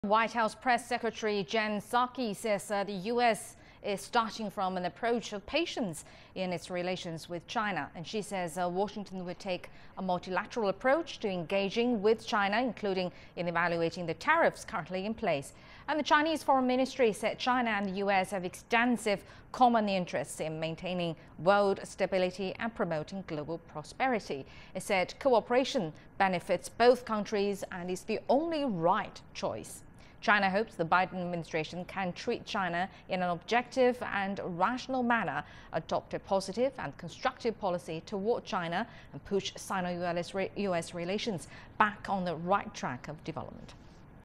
Chinese Foreign Ministry says cooperation benefits both China and U.S.
White House Press Secretary Jen Psaki said on January 25 that the U.S. will take a multilateral approach to engaging with China, including evaluating the tariffs currently in place. The next day, the Chinese Foreign Ministry said that China and the U.S. have an extensive common interest in maintaining world stability and promoting global prosperity.